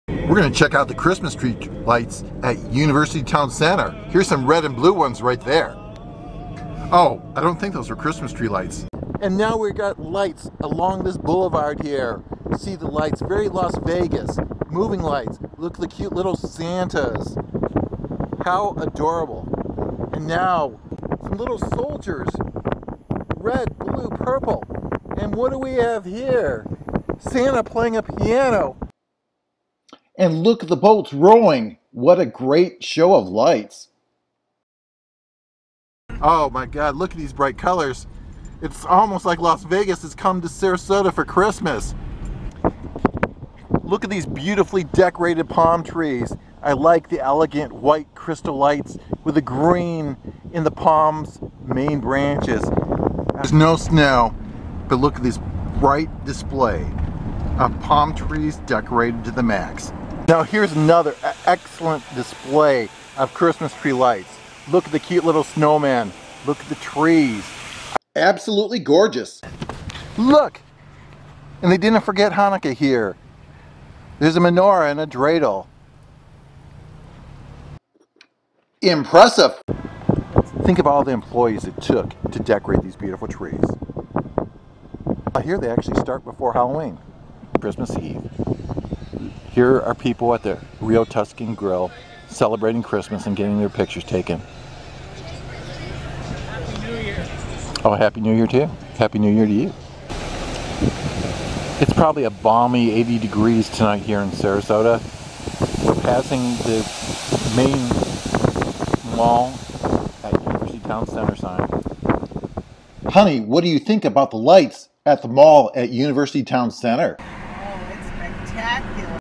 UTC Mall Holiday Lights - A Dazzling Display in Sarasota, FL! ✨
Join us as we explore the spectacular holiday light display at UTC Mall in Sarasota, Florida! 🌟 In this video, we'll take you on a magical tour of the dazzling lights and festive decorations that make this shopping center a must-visit destination during the holiday season. The Mall at University Town Center is located at 140 University Town Centre Drive, Sarasota, Fl 34243. Don't forget to give this video a thumbs up 👍 if you enjoyed our festive adventure, and if you haven't already, hit that subscribe button to stay updated on all our latest adventures in Florida's most beautiful locations! 🔔UTCMallHolidayLights